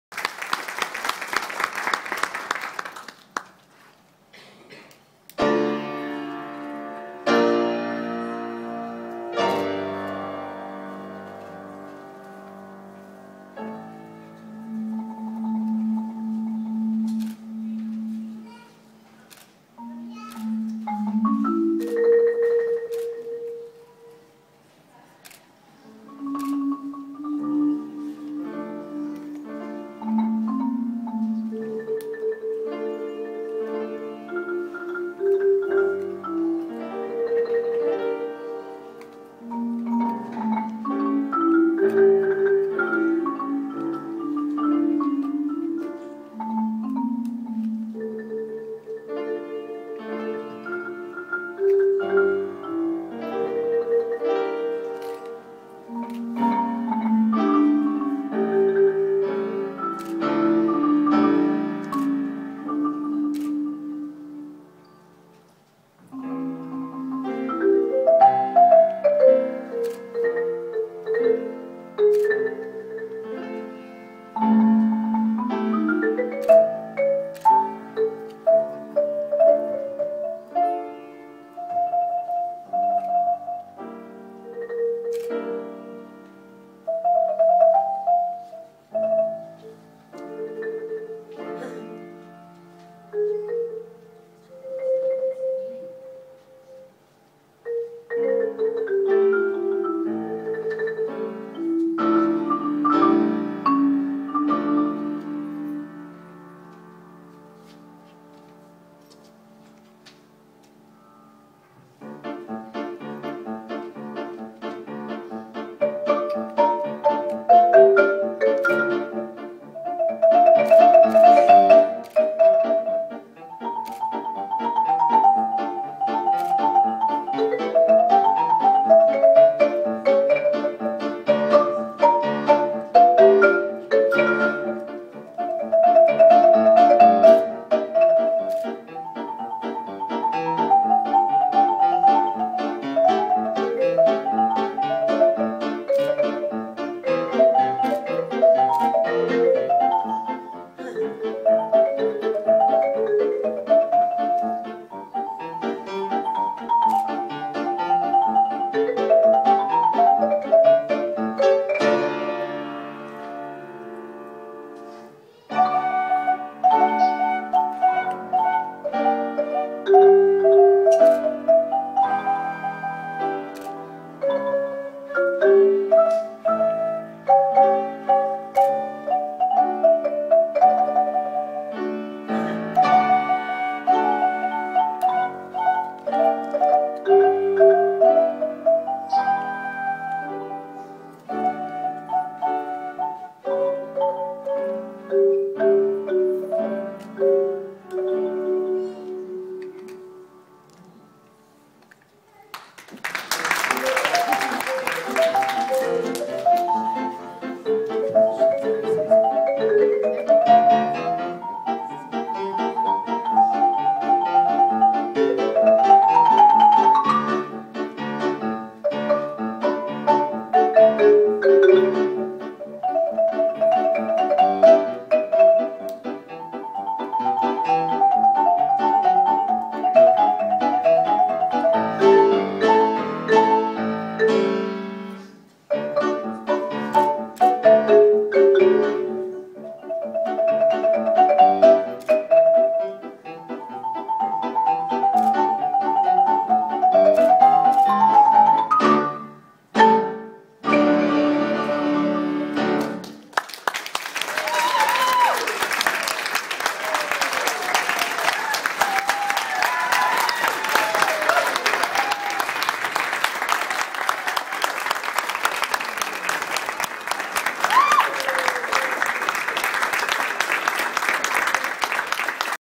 Mathew plays the Marimba
Mathew Lu plays the Marimba beautifully at the Albinism Fellowship of Australia conference 2013 in Sydney.

He is accompanied by his Mum, Yan-ling, and sister Sandy.